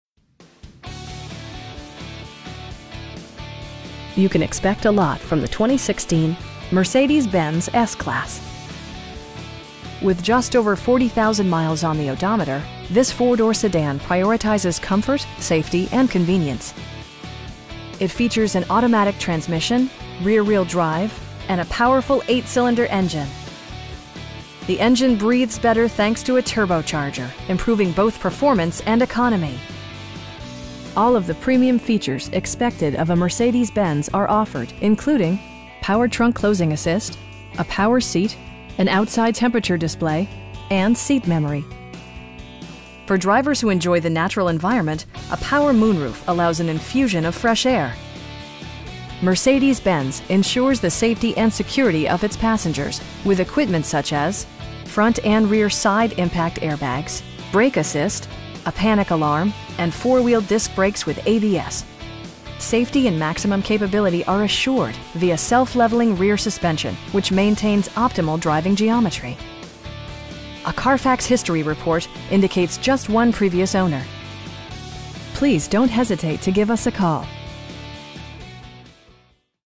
2016 Mercedes-Benz S-Class S 550 in Santa Monica, CA 90404
Major Motor Cars

You can expect a lot from the 2016 Mercedes-Benz S-Class. With just over 40,000 miles on the odometer, this 4 door sedan prioritizes comfort, safety and convenience. It features an automatic transmission, rear-wheel drive, and a powerful 8 cylinder engine. The engine breathes better thanks to a turbocharger, improving both performance and economy. All of the premium features expected of a Mercedes-Benz are offered, including: power trunk closing assist, a built-in garage door transmitter, a power seat, an outside temperature display, automatic dimming door mirrors, heated and ventilated seats, and seat memory. For drivers who enjoy the natural environment, a power moon roof allows an infusion of fresh air. Mercedes-Benz ensures the safety and security of its passengers with equipment such as: dual front impact airbags, front and rear side impact airbags, traction control, brake assist, anti-whiplash front head restraints, a panic alarm, an emergency communication system, and 4 wheel disc brakes with ABS. Safety and maximum capability are assured via self leveling rear suspension, which maintains optimal driving geometry. A Carfax history report indicates just one previous owner. We pride ourselves in the quality that we offer on all of our vehicles. Please don't hesitate to give us a call.